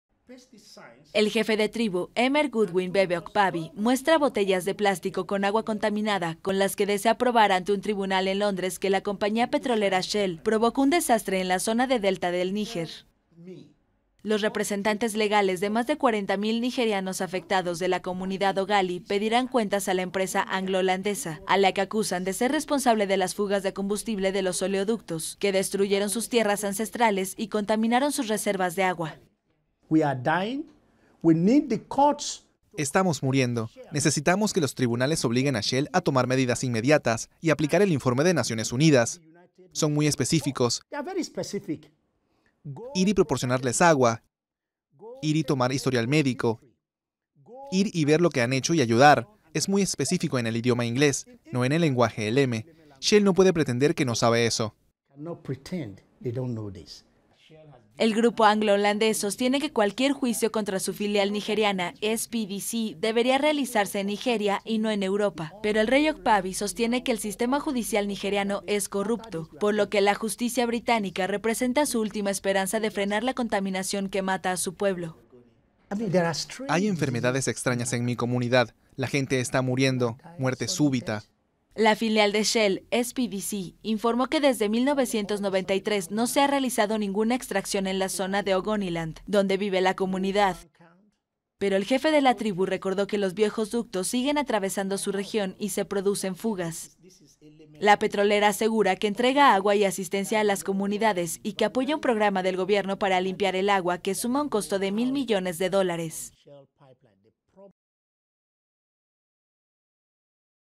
Nigerianos llevan a Shell a la corte por contaminación del agua
El jefe de tribu Emere Godwin Bebe Okpabi muestra botellas de plástico con agua contaminada con las que desea probar ente un tribunal en Londres que la compañía petrolera Shell provocó un desastre en la zona de delta del Níger.